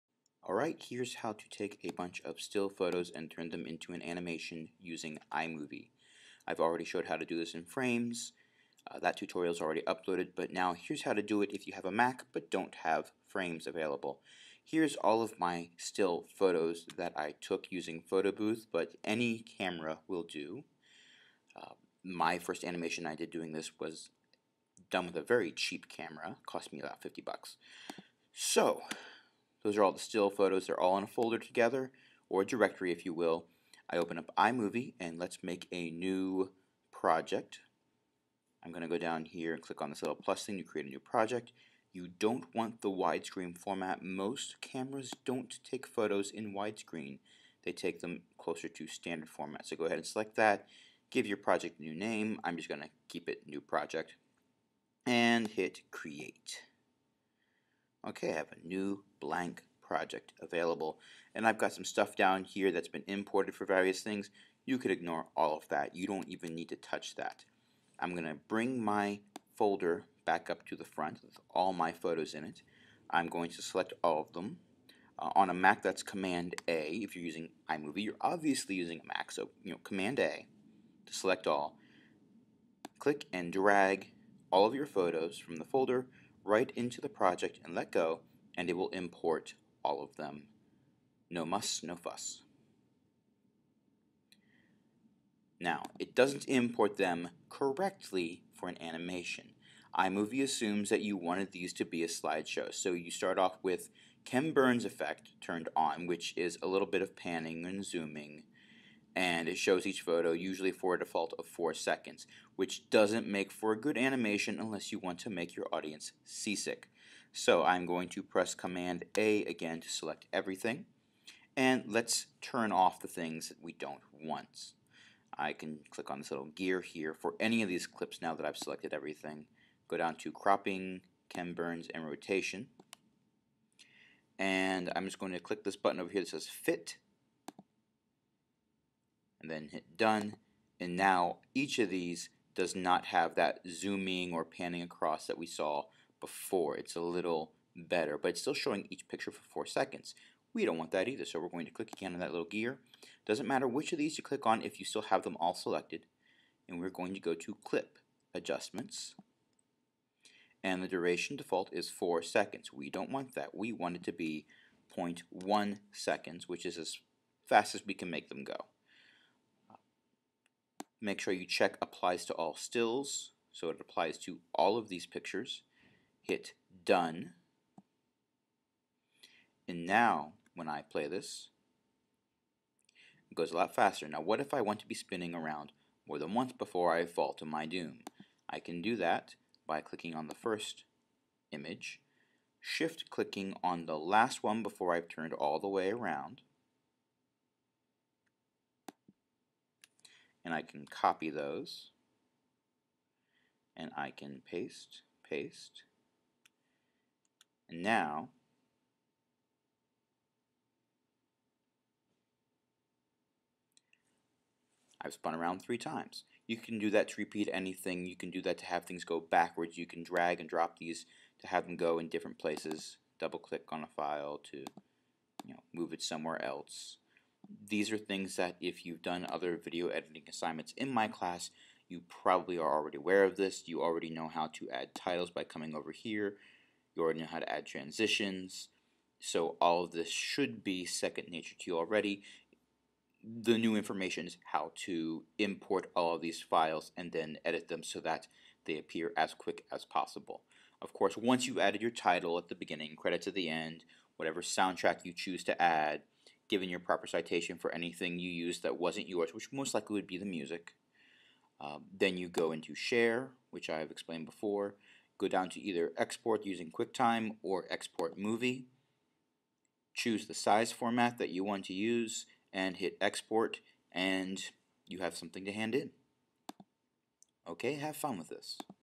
Animation from still photos using iMovie
A short tutorial on how to take a collection of still photos and turn them into a quick and dirty animation using iMovie.